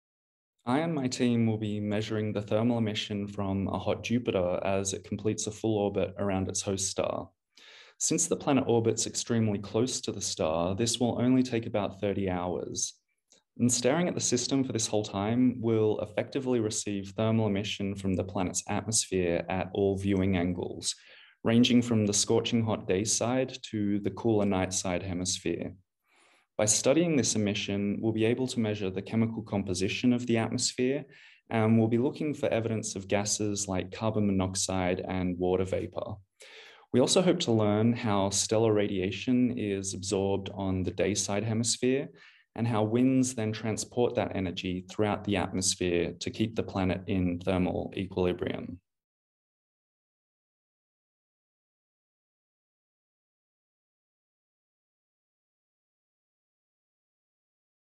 Thomas Mikal-Evans (MPIA) on his research with the James Webb Space Telescope
Thomas Mikal-Evans from Australia, a research group leader in the APEx department at the Max Planck Institute for Astronomy, describes his upcoming research projects with the James Webb Space Telescope. He is going to study the atmospheric processes of the hot Jupiter WASP-121 b, an exoplanet with two distinct hemispheres that either face the central star or the cold and dark space.